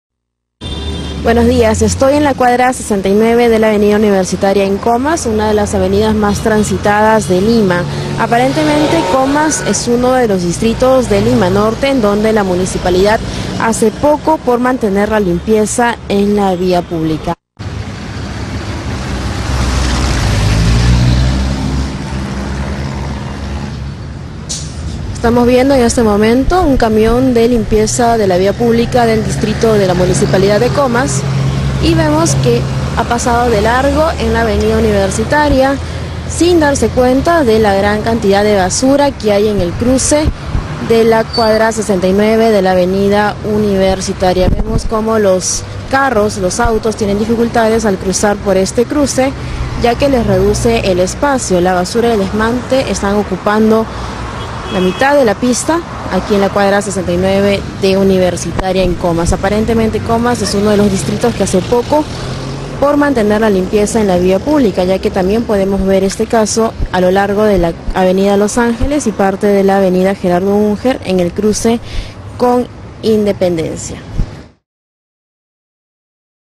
Acopio de basura en Comas
LIMA.- La cuadra 69 de la avenida universitaria se ha convertido en un botadero de basura. En la mañana un camión de limpieza de la Municipalidad de Comas pasó por allí y no hizo nada. Chavely Chiroque con el informe.